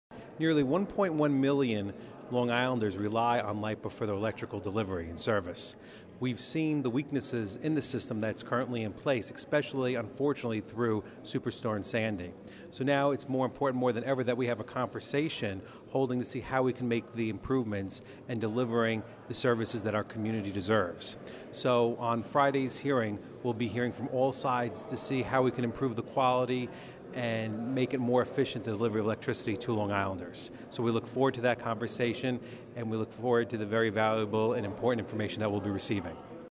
NYS Assemblyman Chad Lupinacci Welcomes Public Input on LIPA
Assemblyman Chad Lupinacci (R,C,WF,I-Huntington Station) welcomes public input on potential reforms to LIPA. Lupinacci also reaffirms his commitment to delivering high-quality services for Long Island.